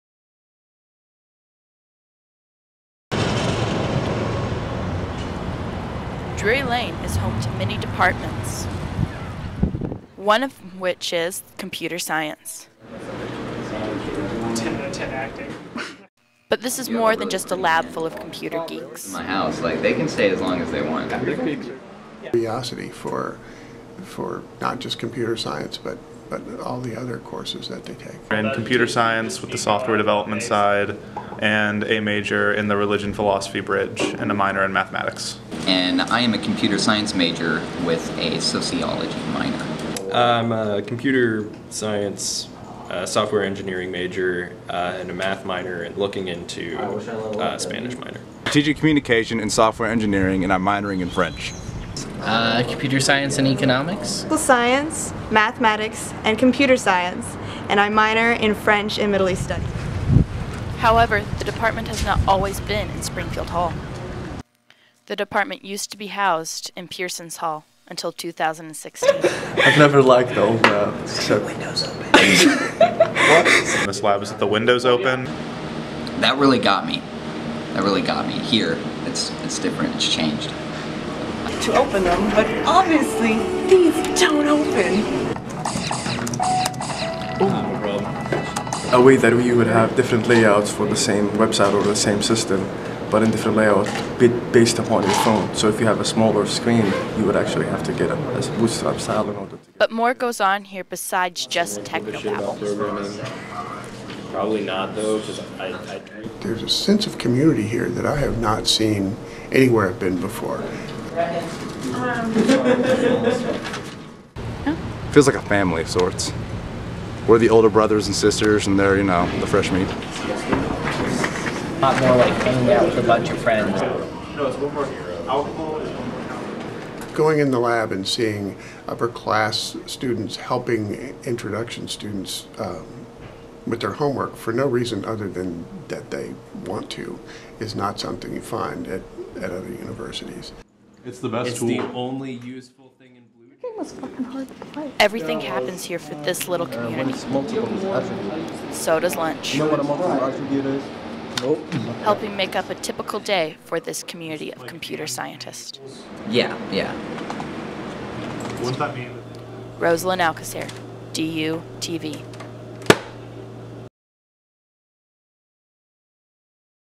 DU Computer Science
This video is about the department of Computer Science at Drury